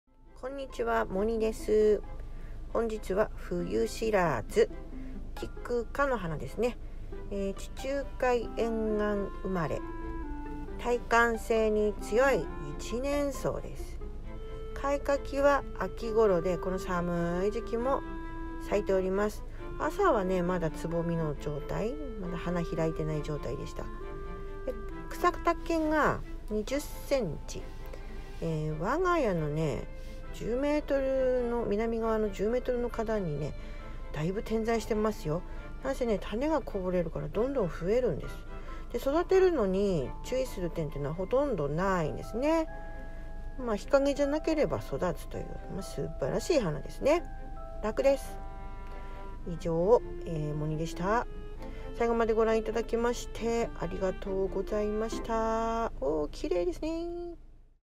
Moniの趣味チャンネル 冬しらずってこんな花なんです！花# 冬しらず
我が家の庭に咲く花の動画です！名前だけでも、覚えてね！